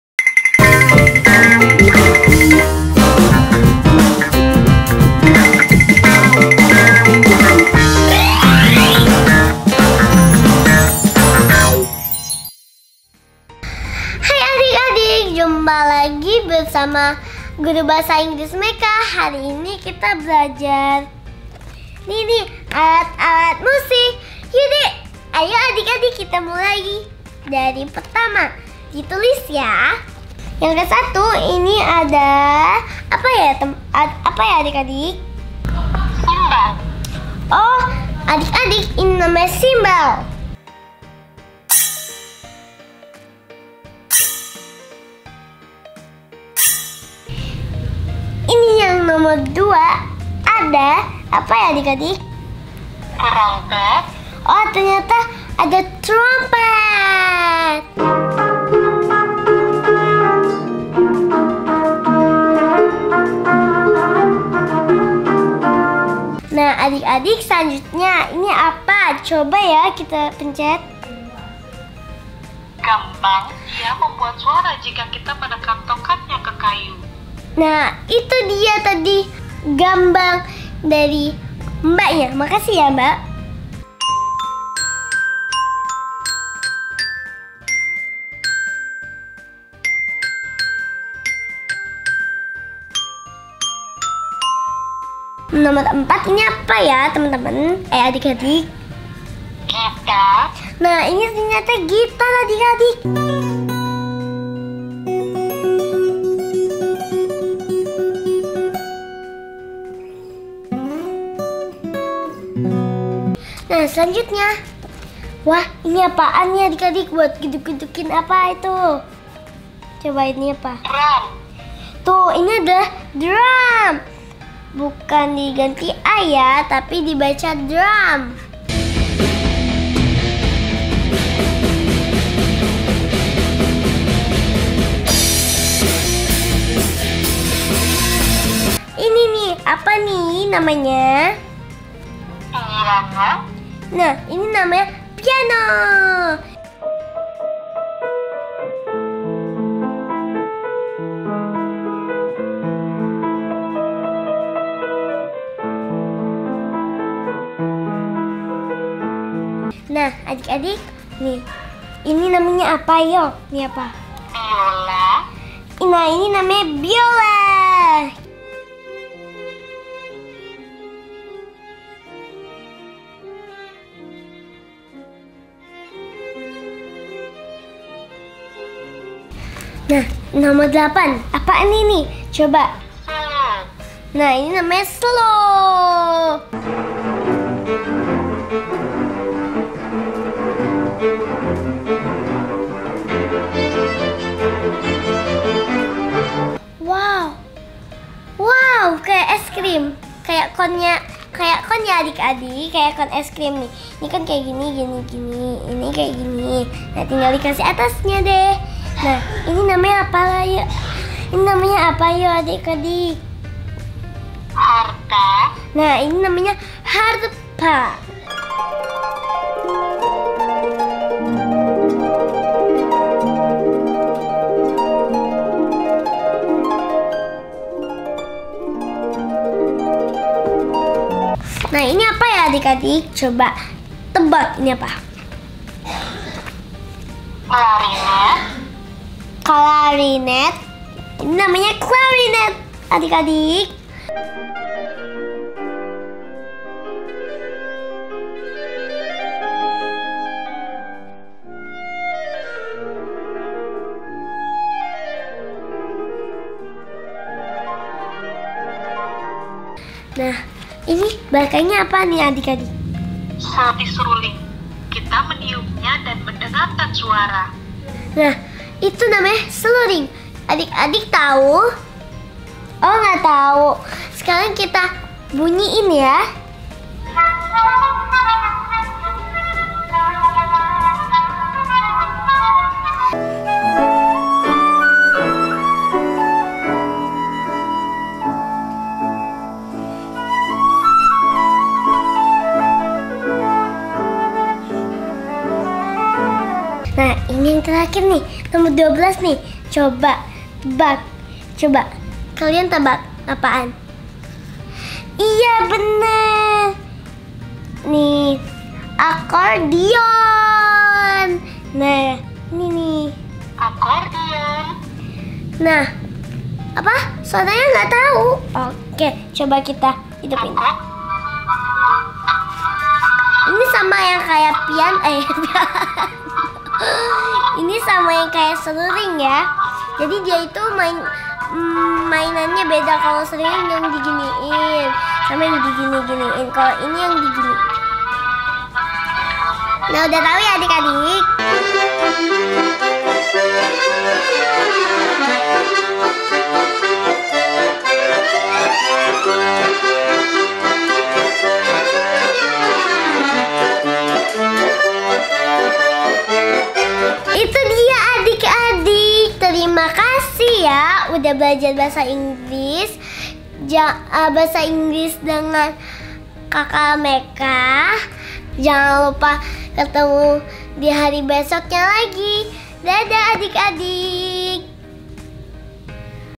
Mengenal Alat Musik untuk anak-anak
Apa saja alat musik yang bisa kita mainkan? nah mecca mau mengenalkan beberapa alat musik yang bisa kita mainkan, ada gitar terompet, piano, biola dan lain-lain